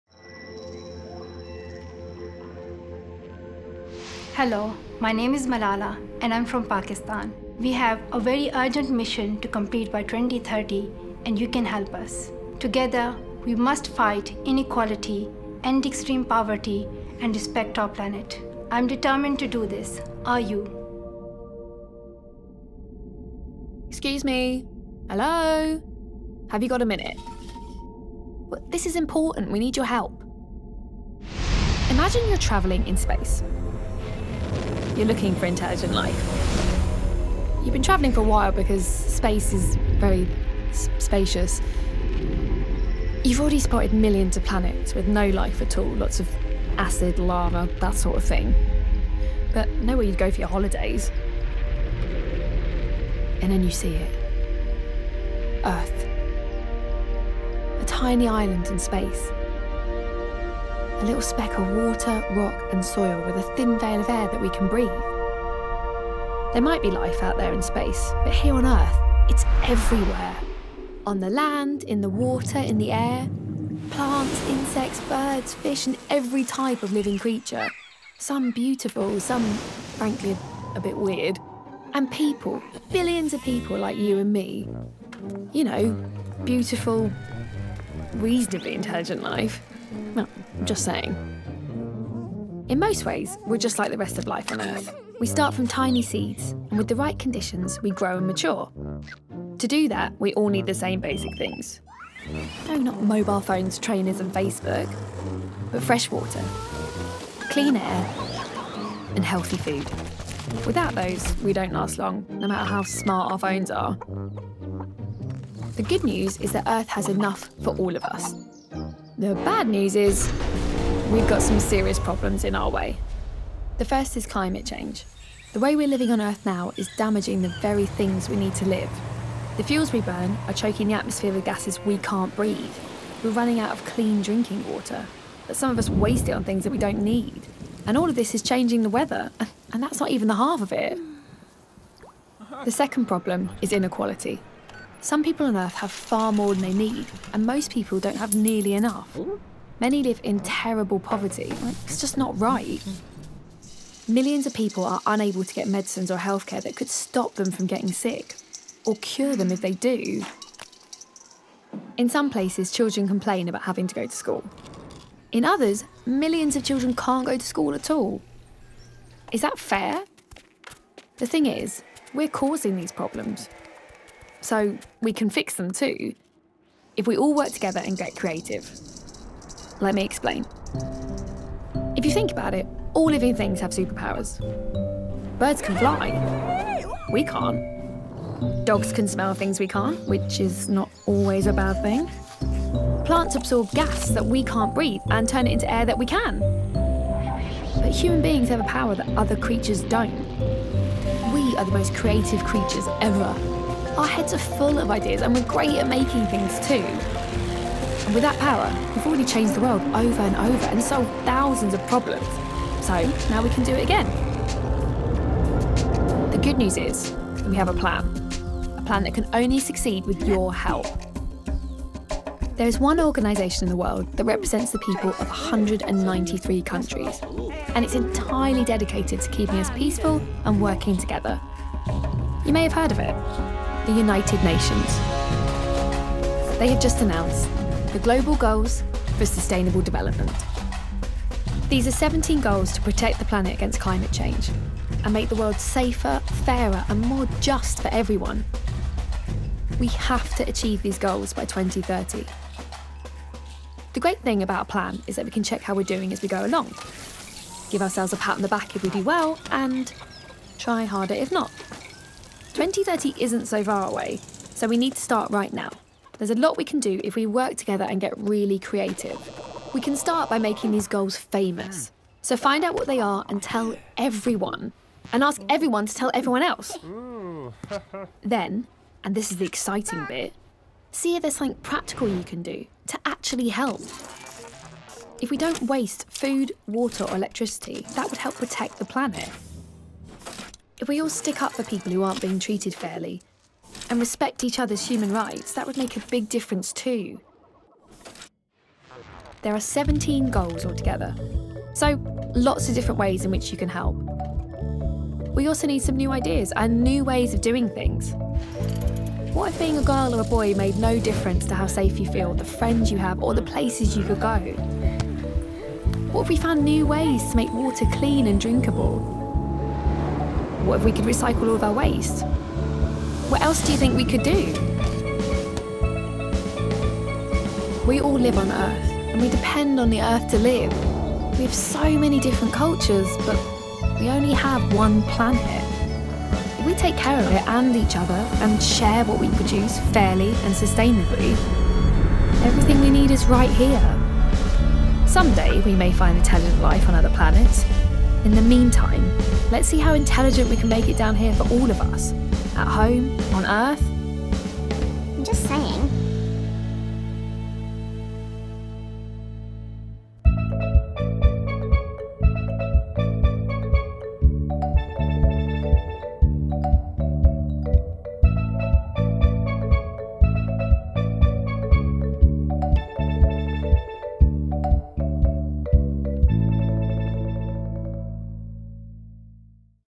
Malala: wprowadzenie do Celów Zrównoważonego Rozwoju
Malala Yousafzai, najmłodsza laureatka Pokojowej Nagrody Nobla, opowiada o wyzwaniach, przed którymi stoi ludzkość, i o tym, jak ważna jest współpraca wszystkich ludzi, aby stawić im czoła. Wprowadza nas w temat Celów Zrównoważonego Rozwoju, dzięki którym stworzymy sprawiedliwszy i bezpieczniejszy świat oraz ochronimy naszą planetę przed zmianą klimatu.

Projekt współfinansowany w ramach programu polskiej współpracy rozwojowej Ministerstwa Spraw Zagranicznych RP.

[ss]